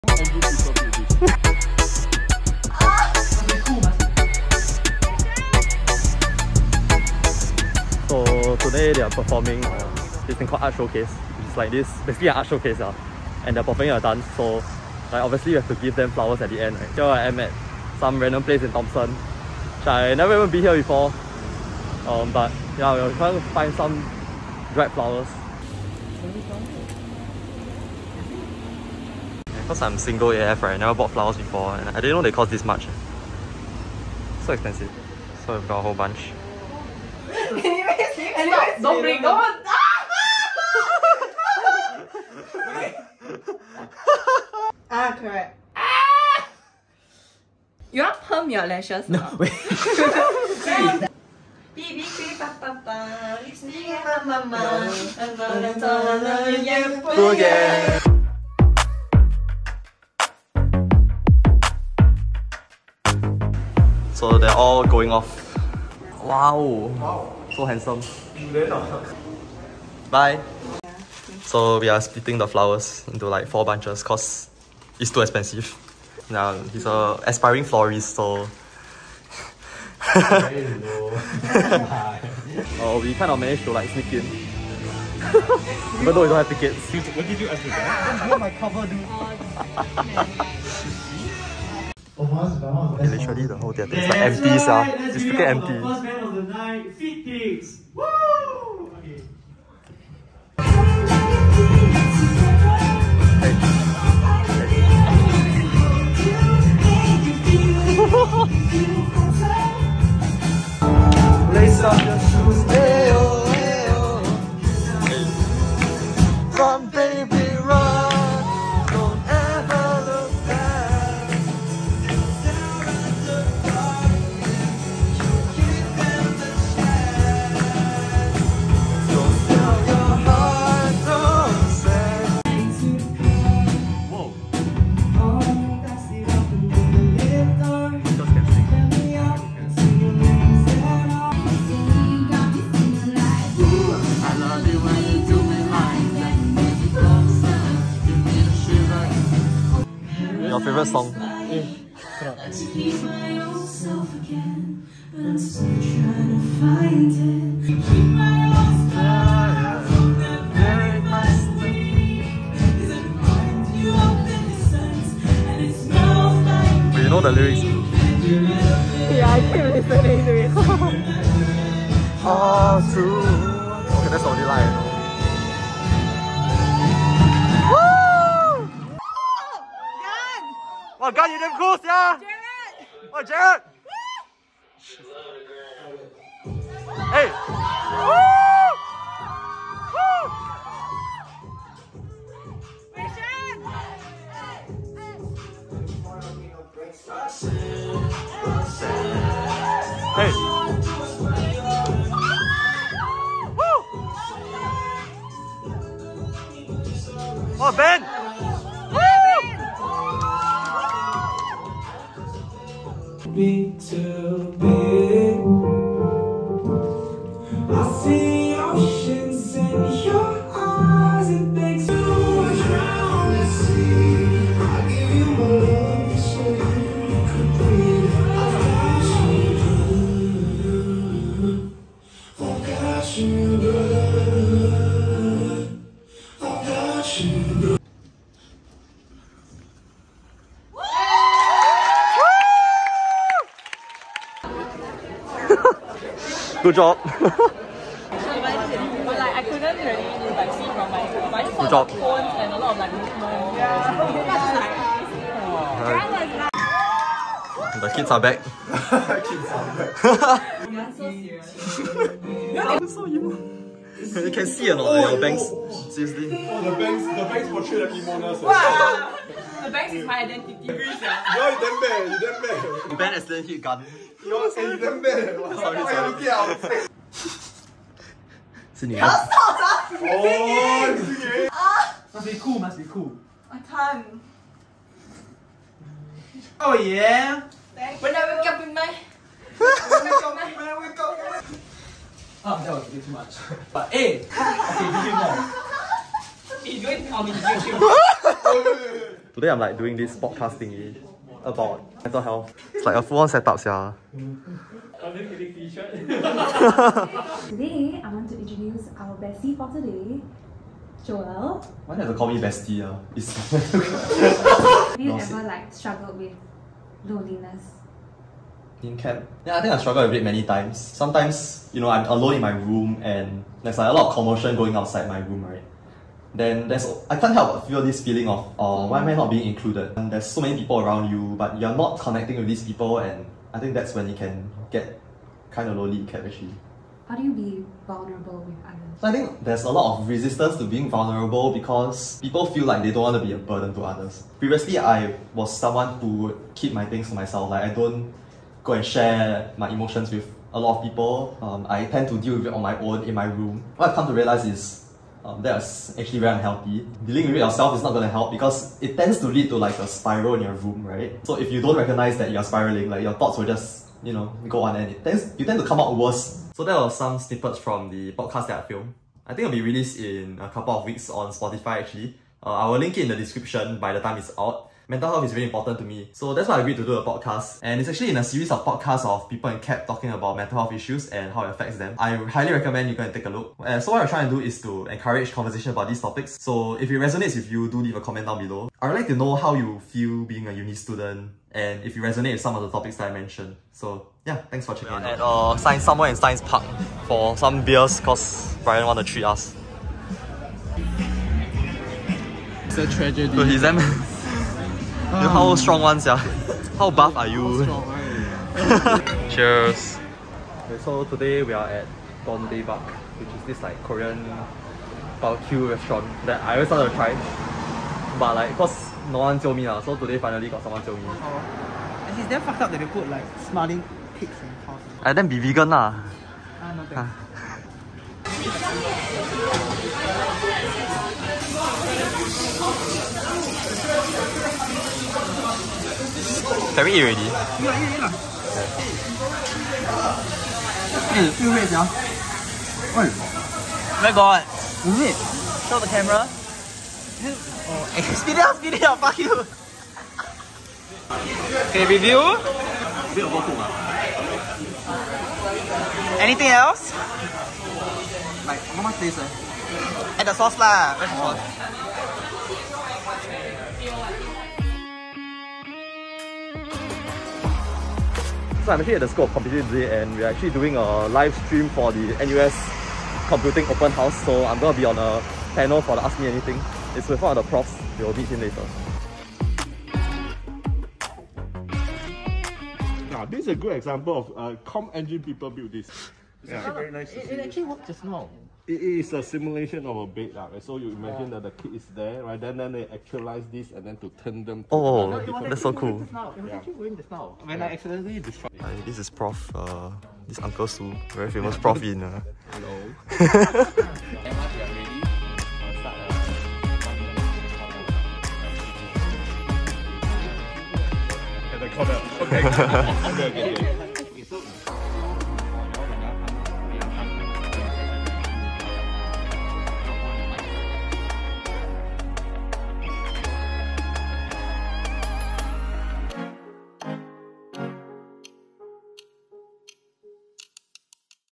First LIVE Concert in NUS | Recess Week Vlog
Recess week shenanigans in NUS. Watched a live music and dance performance, filmed a podcast about mental health and did other random things.

Podcast will be released soon!